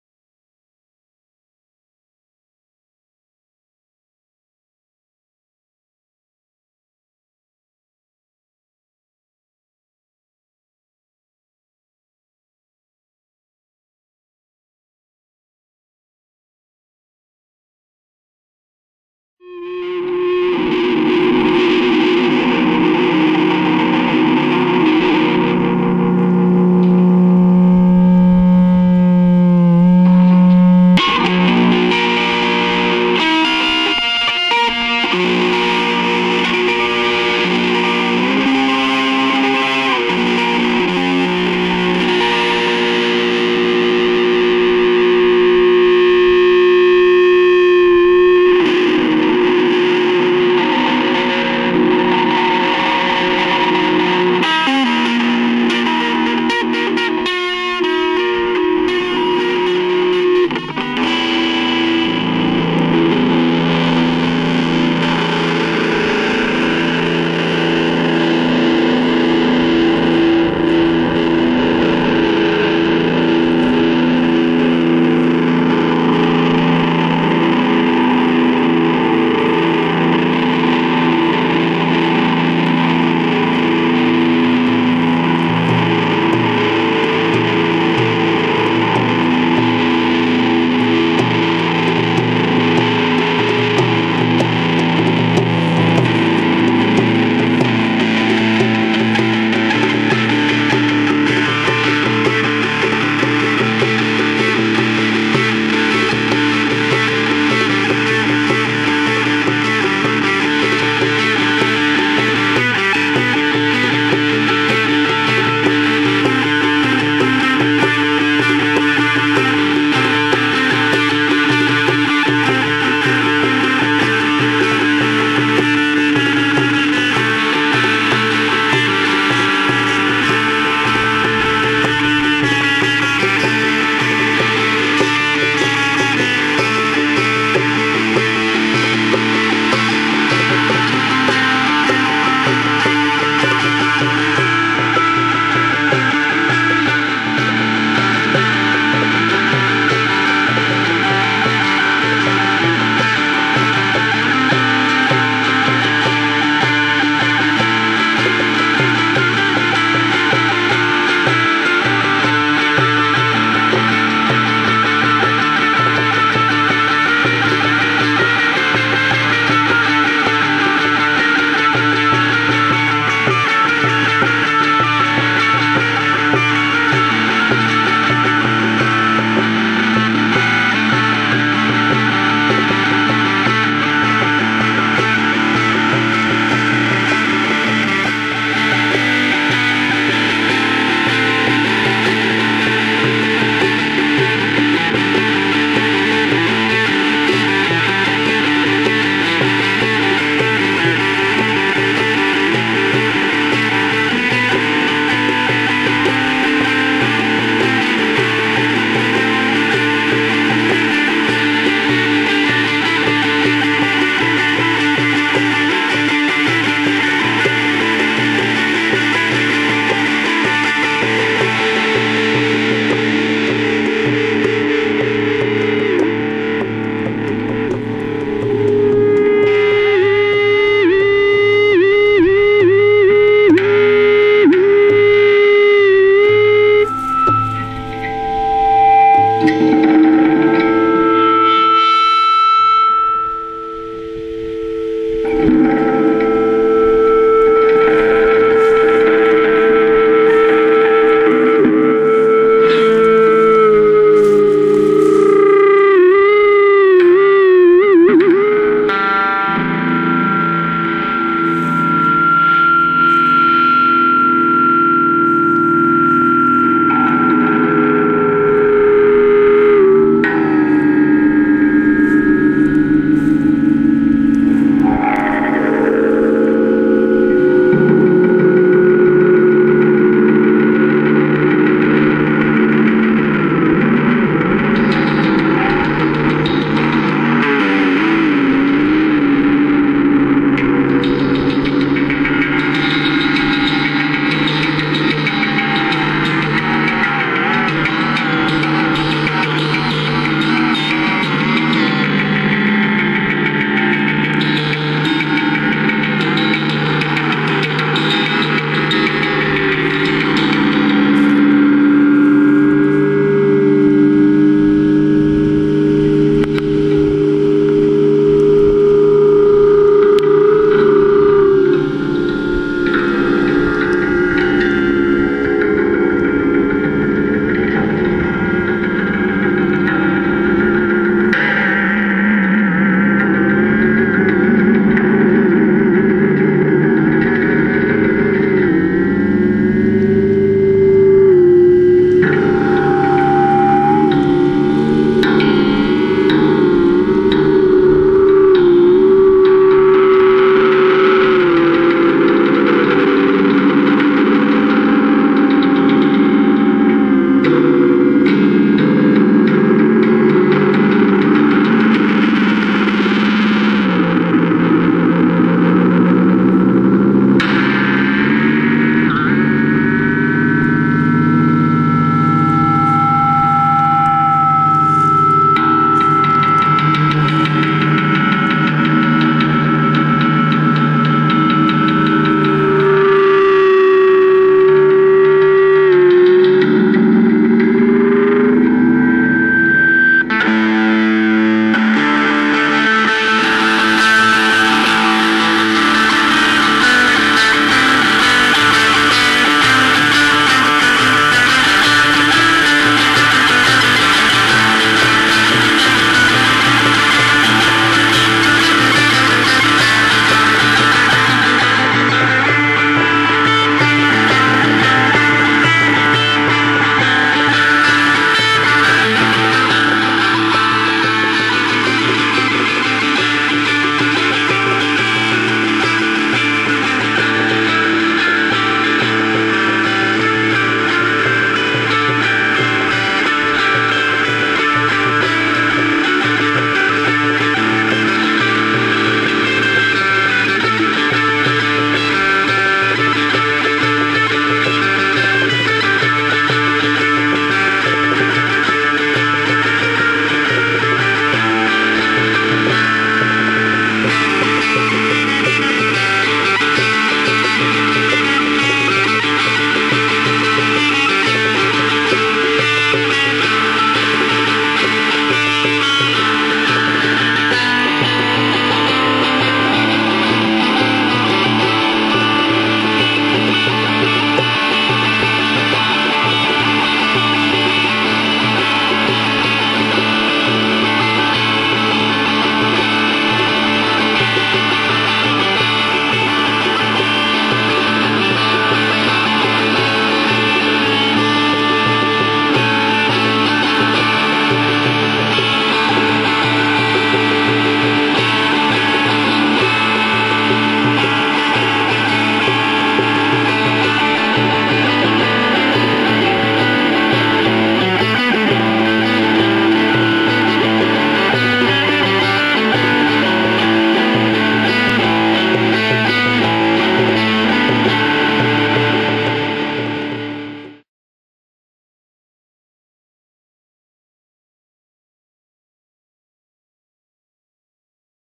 Transdimensional Broadcast
live inside the last crop circle (a hypercube) of 2007
in the wiltshire plains